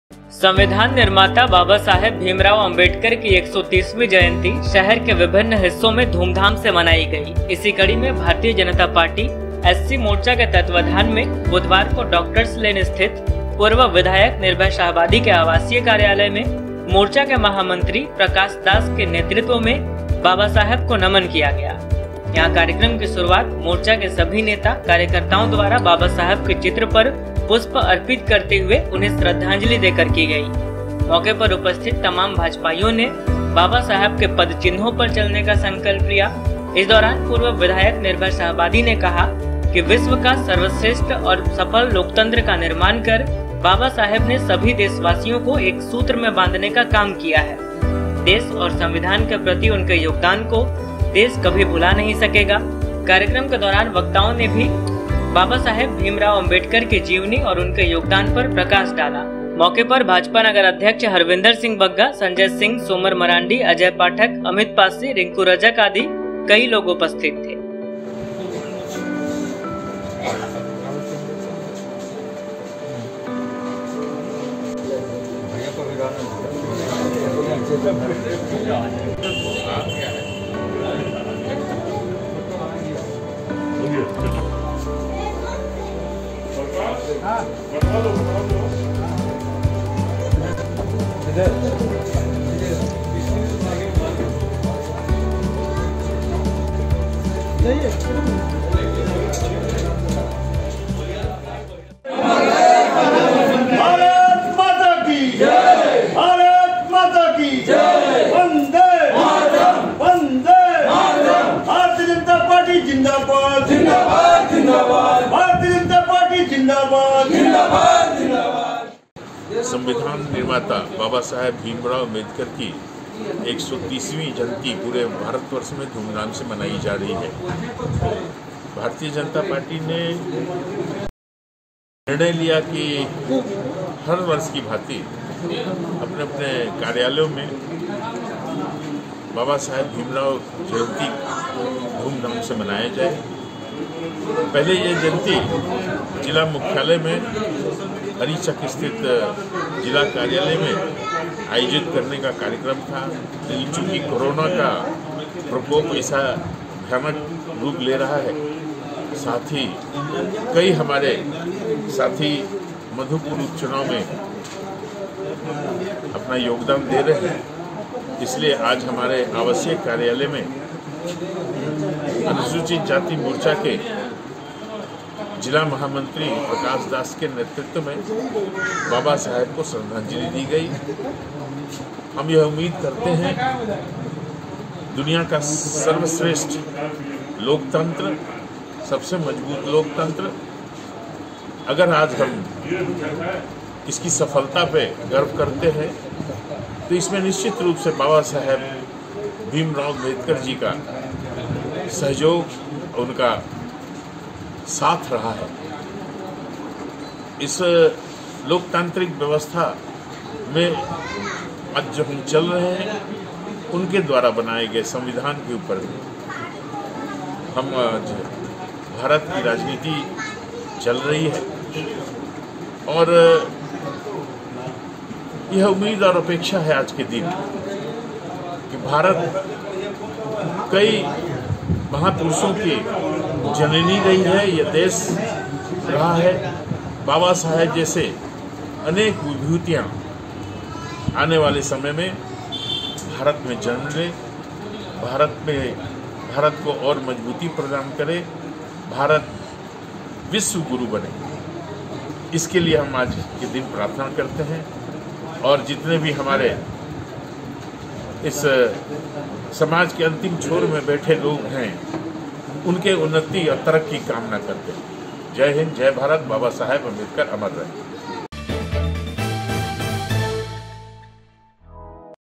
Giridih पूर्व विधायक के आवासीय कार्यालय में हर्षोल्लास के साथ भीमराव अंबेडकर की जयंती मनाई गई
भारतीय जनता पार्टी एससी मोर्चा के तत्वाधान में भाजपा के पूर्व विधायक निर्भय शाहाबादी के मकतपुर स्थित आवासीय कार्यालय में हर्षोल्लास के साथ संविधान निर्माता बाबा साहेब भीमराव अंबेडकर की 130वी जयंती मनाई गई।इस दौरान वक्ताओं ने उन्हें युगपुरुष बताया और उनकी चित्र पर पुष्प अर्पित कर उन्हें श्रद्धांजलि अर्पित की।